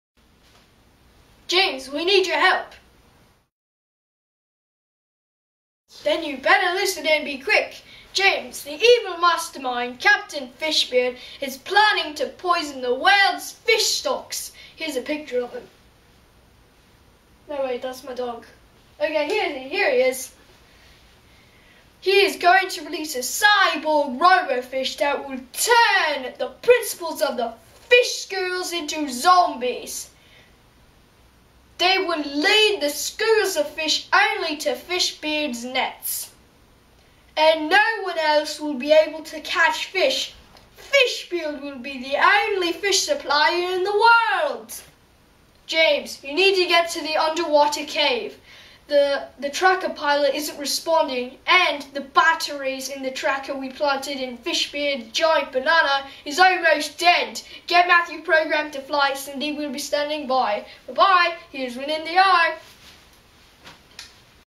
One more day of filming on Saving the world before Lunch
T-man finished his part. He's in his secret layer relaying information to James and Mathew about the mission. The random" dancers was a James idea. :)